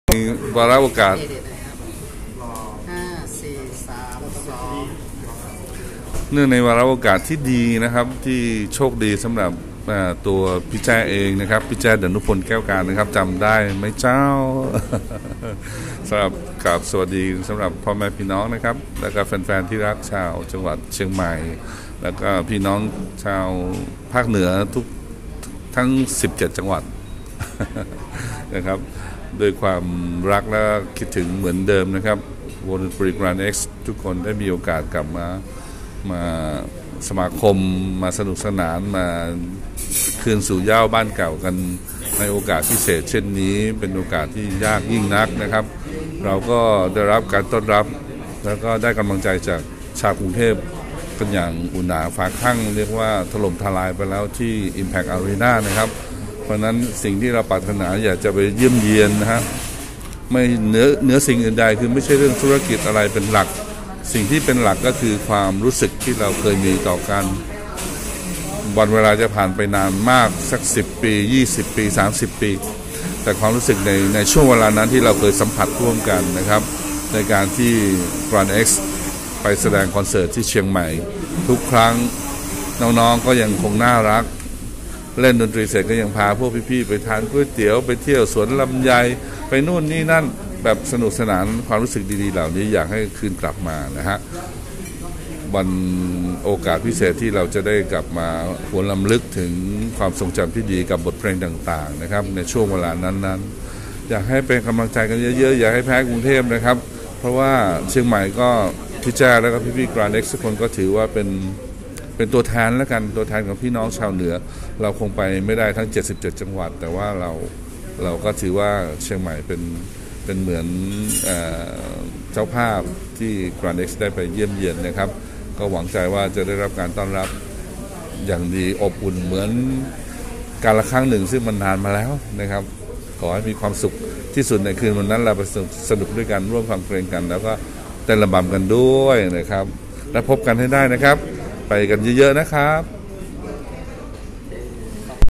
พี่แจ้ ดนุพล ชวนไปสนุกด้วยกัน “วงแกรนด์เอ็กซ์” ทัวร์คอนเสิร์ตที่เชียงใหม่ 27 ส.ค.59
สัมภาษณ์ พี่แจ้ ดนุพล เชิญชวนไปสนุกด้วยกัน “วงแกรนด์เอ็กซ์” ทัวร์คอนเสิร์ตที่เชียงใหม่  27 ส.ค.59